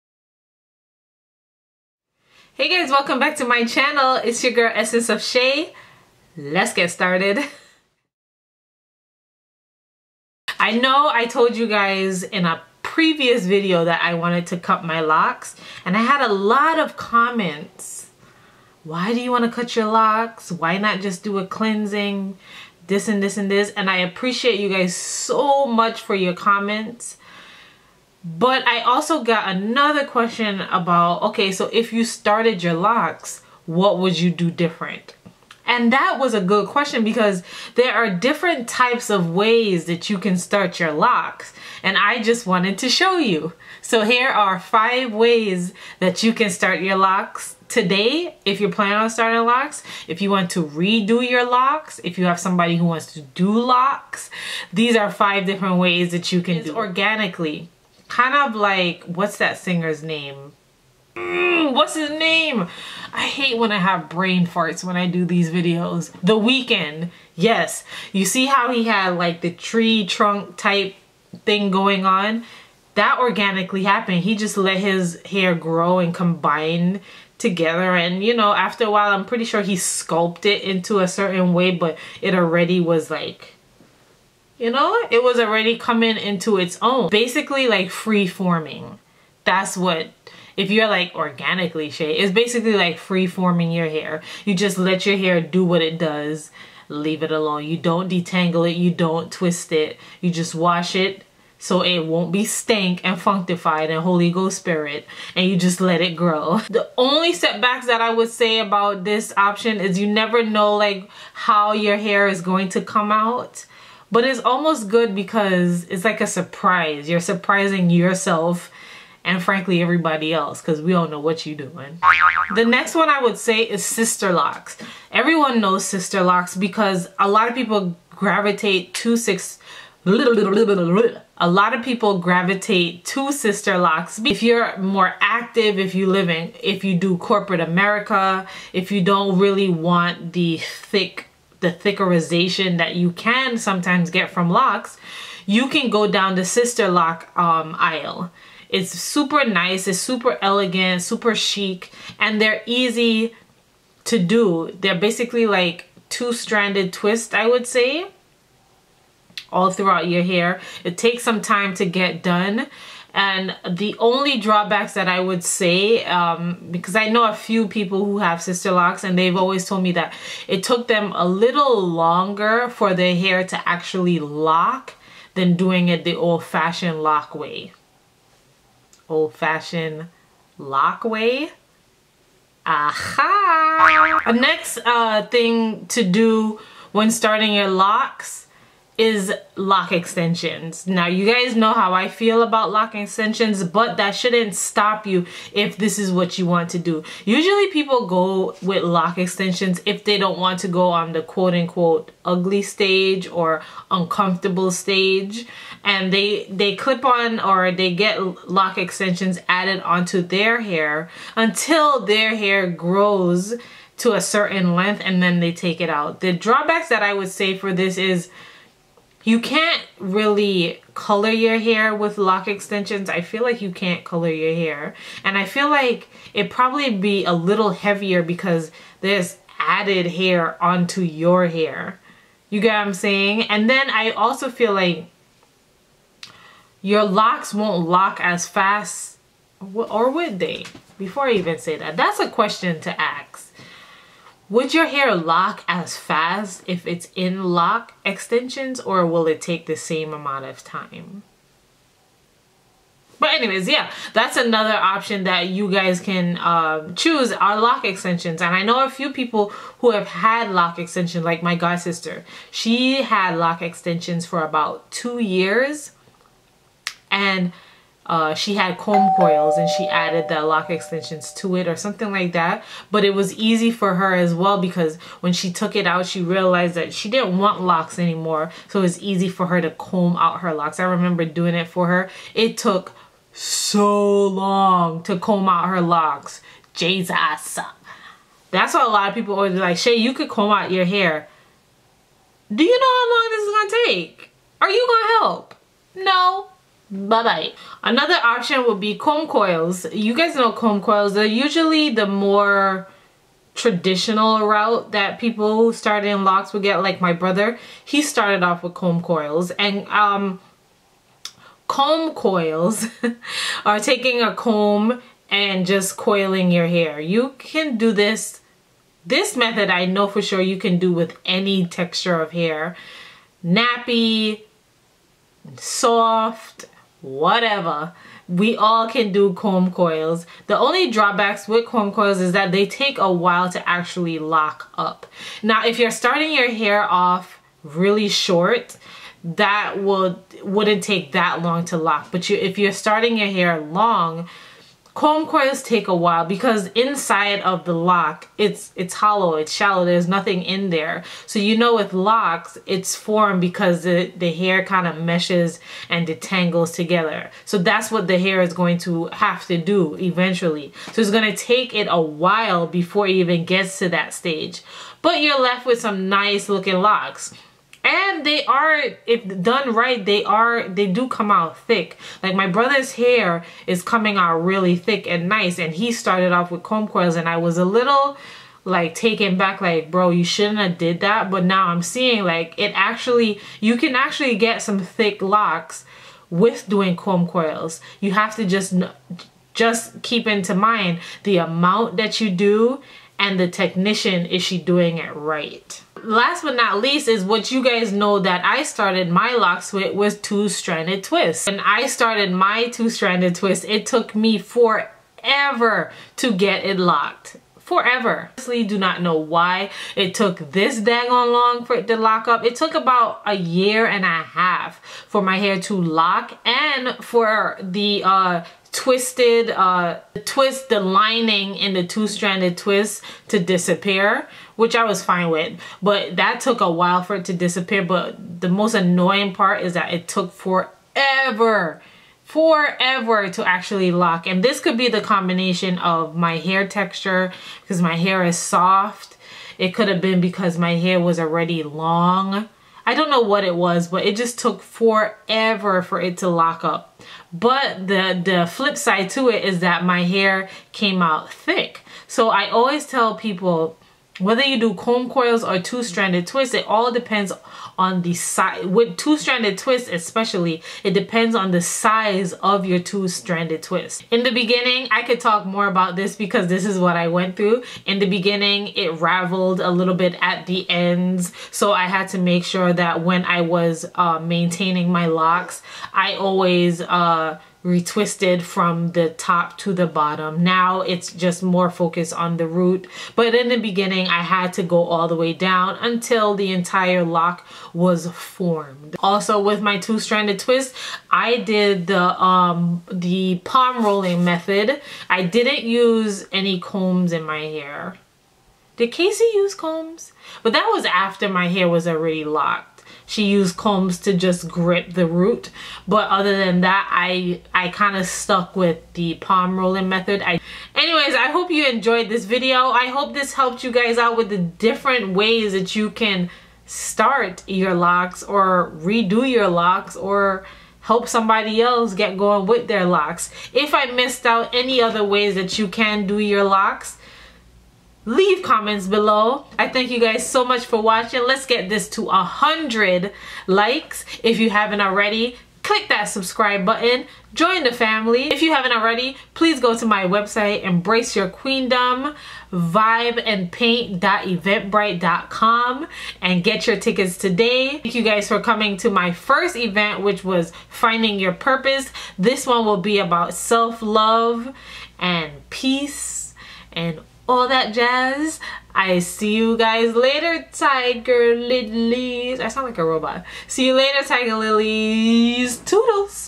5 WAYS TO START OR RESTART YOUR LOCS I ESSENCEOFSHAY
Hey Guys! WATCH ON HD!!! Hi my loves I am back with another video for ya! This one is all about the different ways to start or re-do your locs! There are soooo many ways but I narrowed it down to five.

1. Freeform
2. Sisterlocks
3. Loc Extentions
4. Comb Coils
5. Two Stranded Twists